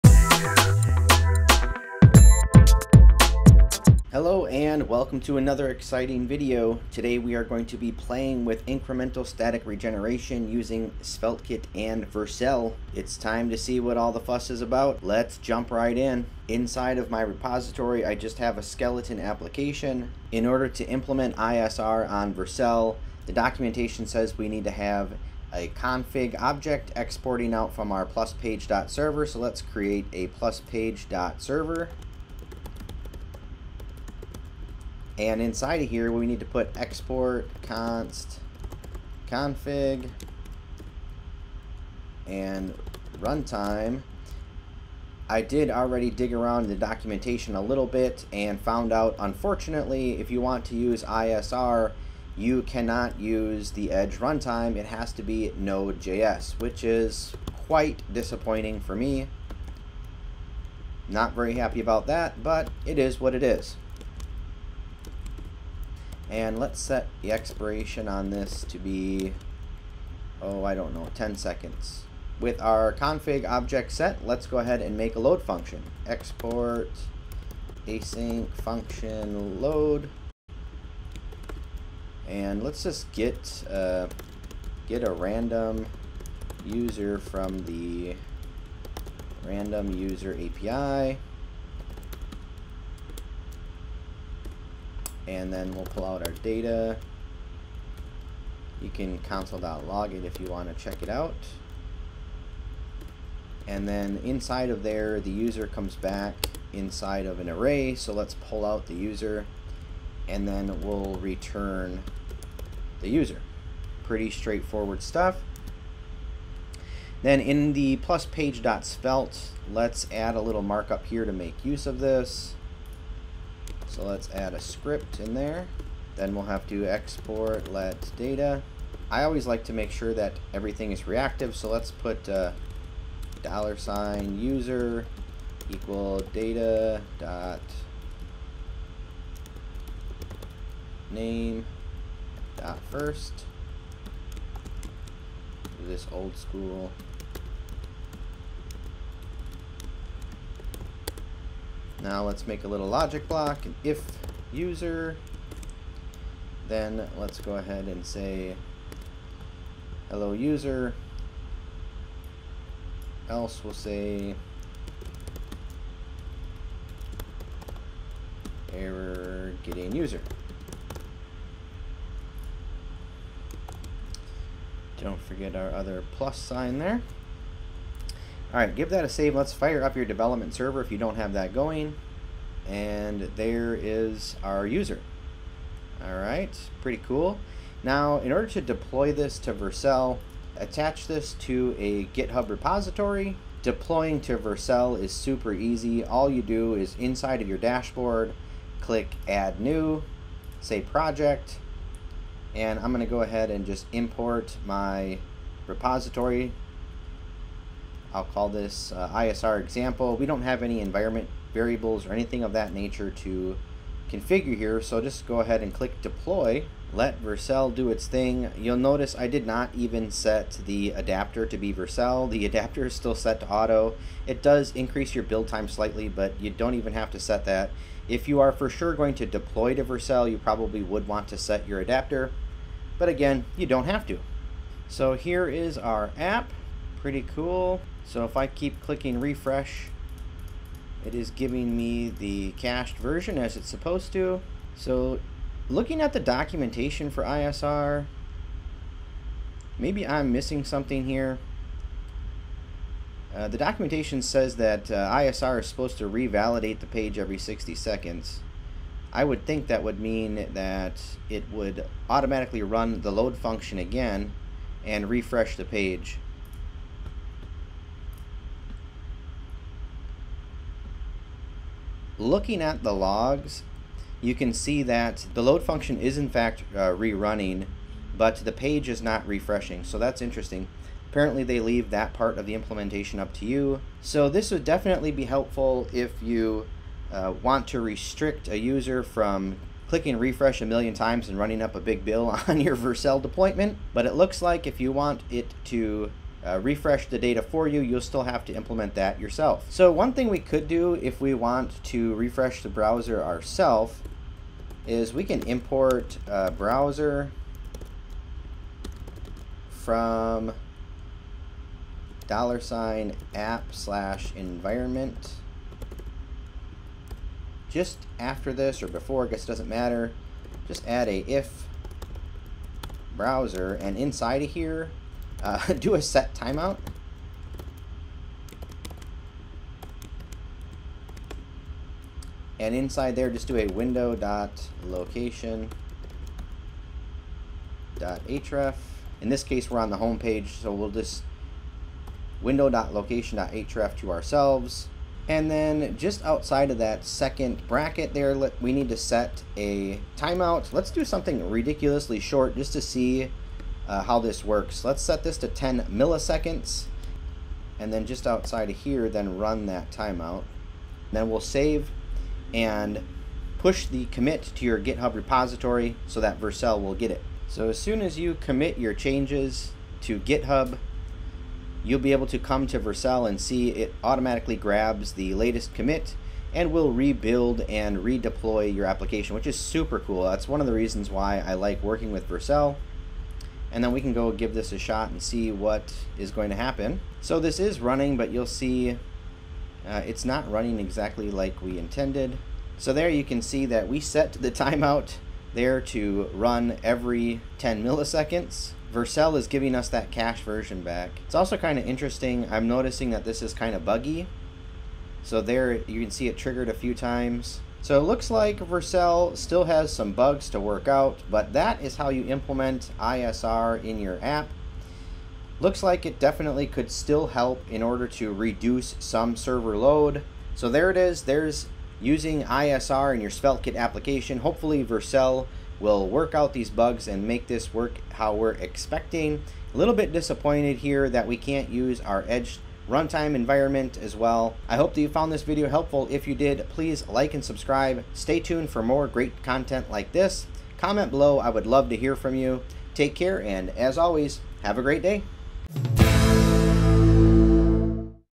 First look at ISR with Vercel / SvelteKit
Vercel just added major features to their SvelteKit deployments.  In this video I setup a very simple app to use ISR and play around with and test it out. I hope you find this video helpful!